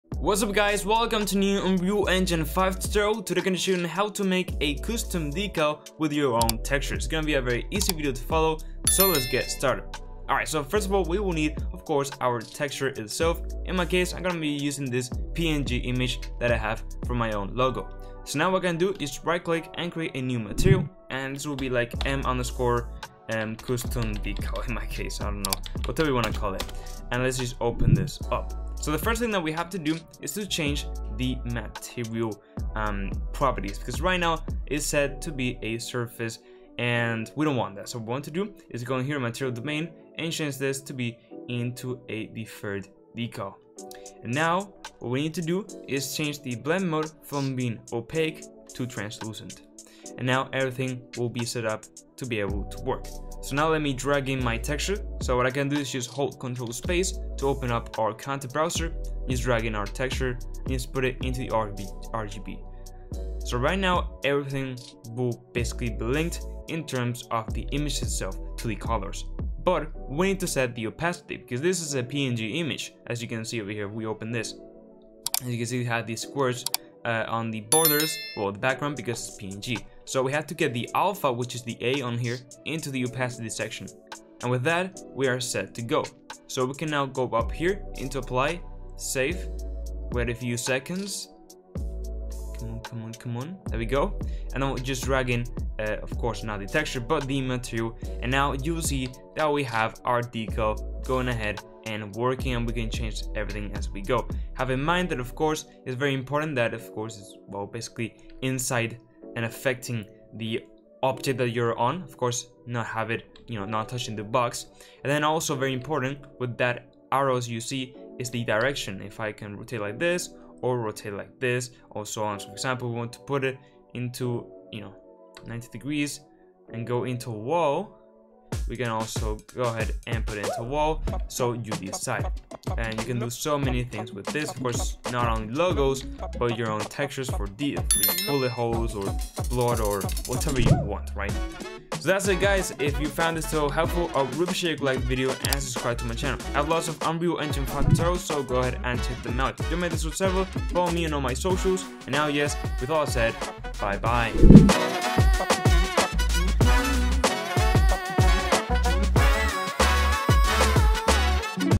How to Make a Custom Decal With a Texture in Unreal Engine 5
Hello guys, in this quick and simple tutorial we are going to learn how to make a custom decal with a texture in Unreal Engine 5!